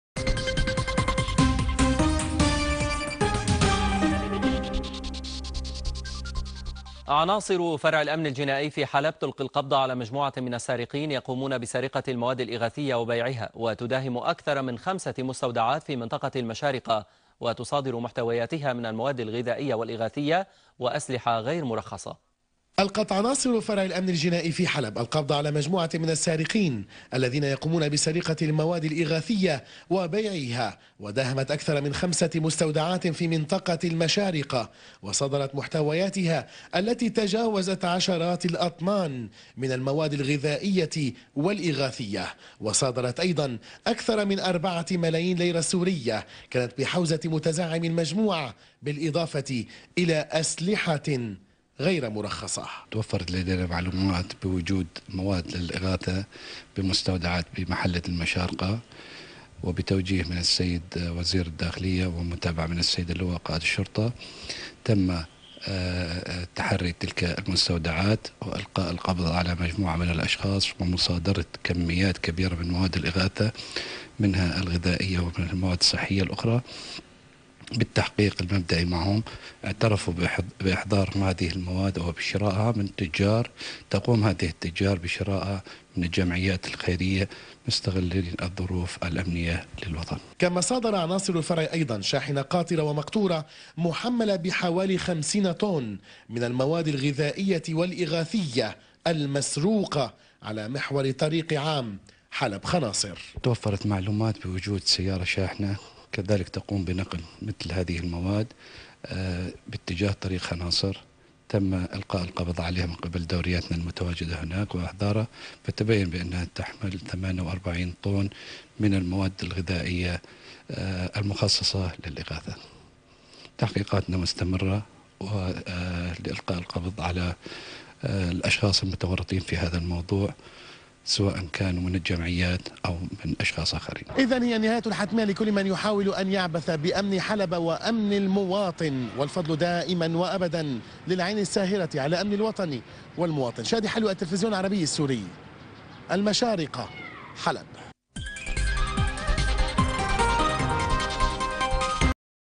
حلب : القبض على مجموعة من السارقين يقومون بسرقة المواد الإغاثية ومن ثم بيعها 25-2-2015
تم الرفع بواسطة : شبكة أخبار حلب الشهباء و الصفحة الرسمية للإعلامي شادي حلوة